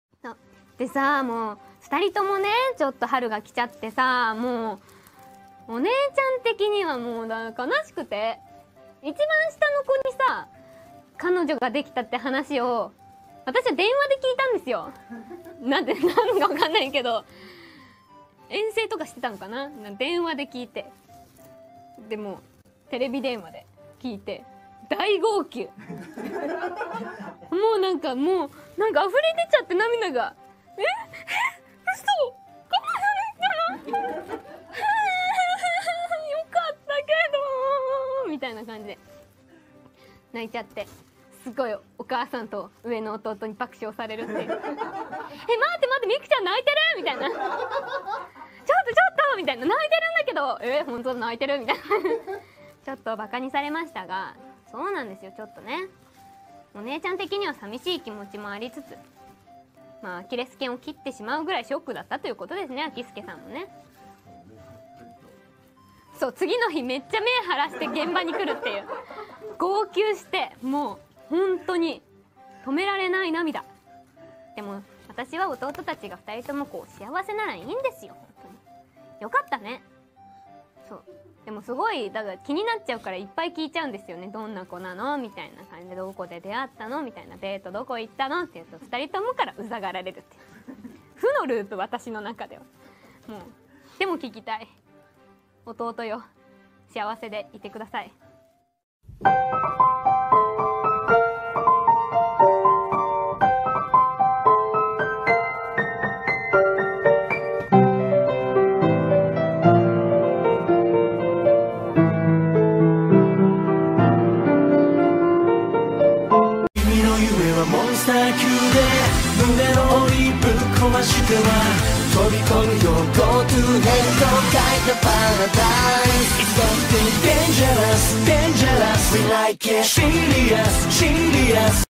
[Eng Sub] Miku Ito's reaction when she found out her brother got a girlfriend
Could find any pics of Miku crying so you get a Go-Toubun pic instead